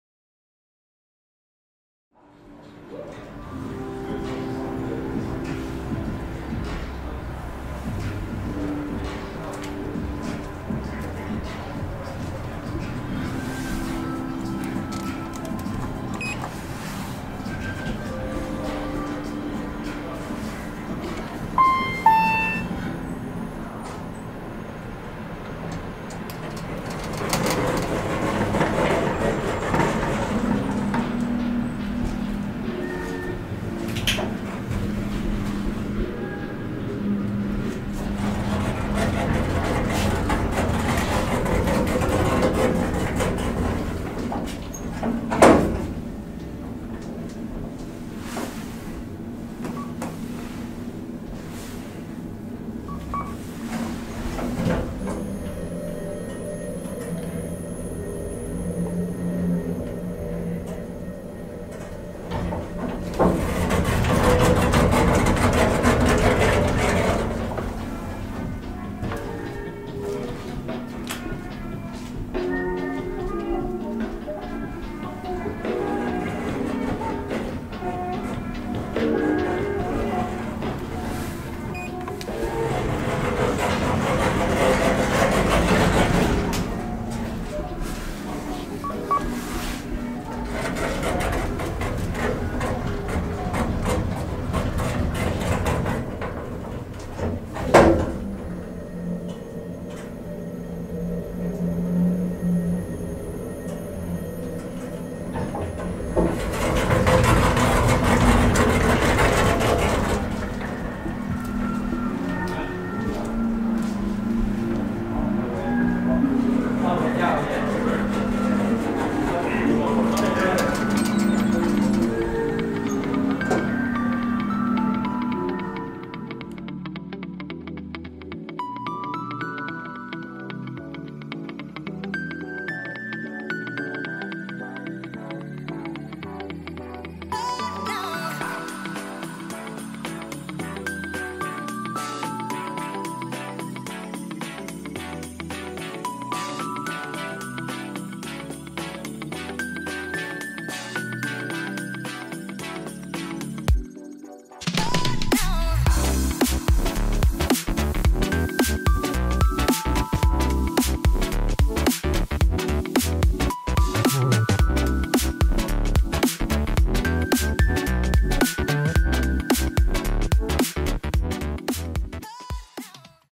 2014 KONE MonoSpace MRL-Traction Elevator @ Ringen Centrum
ELEVATOR INFORMATION!

Manufacturer/Tillverkare: KONE
Model/Modell: MonoSpace
Installation year/Byggår: 2014
Floors/Antal våningar: 3: (-1) 0 1

Location/Adress: Ringen Centrum | Ringvägen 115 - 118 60 Stockholm (Södermalm) SWEDEN!